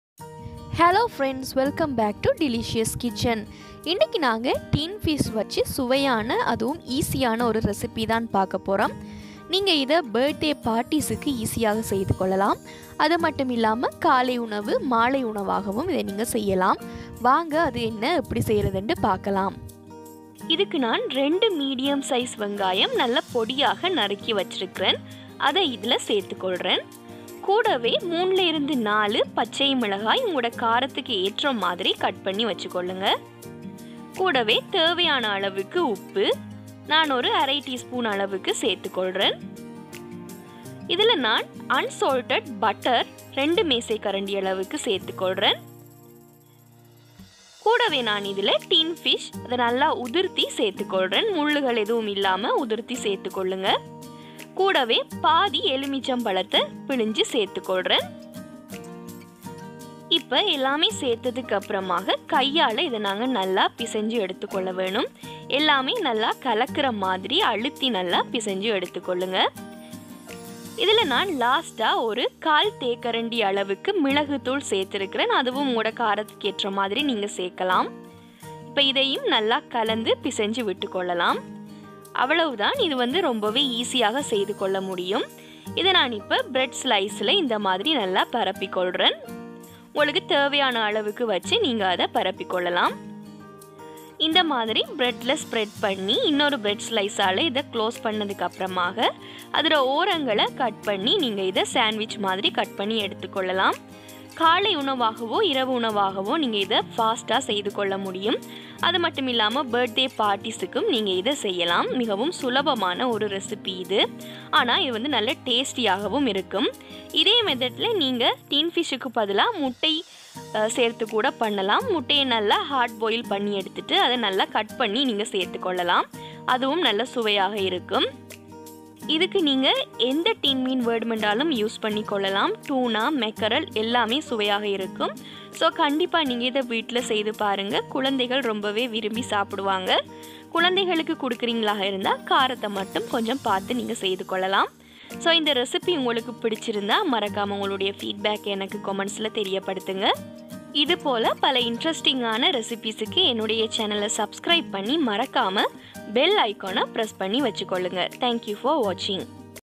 ✔️10 Mins Easy and tasty Sandwich recipe in Tamil (ENG sub) ❤️ | Tuna sandwich| Tin Fish |Delicious
In this video iam sharing most easiest and tasty sandwich for your breakfast, dinner or birthday party. you can make this bread sandwich with very few ingredients within five minutes.

ingredients:
canned fish 1
onion 2
green chillies 3
lime 1/2
pepper powder 1/4tsp
salt
butter 2tbsp